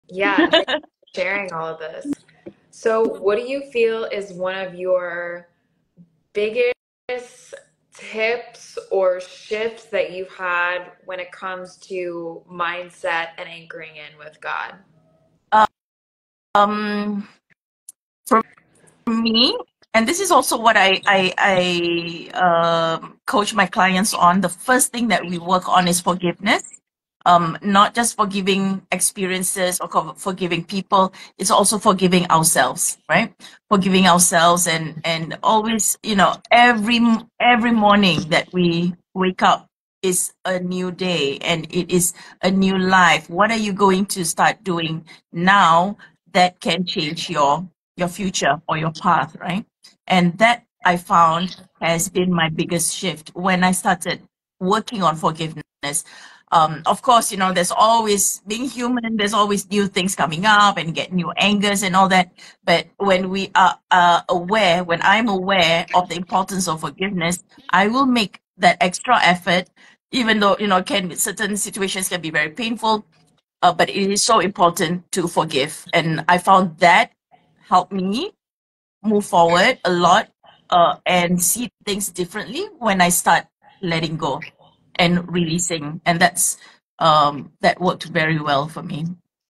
On Forgiveness
Part 5 : Forgiveness

Mallory from Verde (USA) and Murshidah from Love & Respect Transforms (Malaysia/Singapore) discuss the power of shifting your mindset, anchoring in with God, and the power of gratitude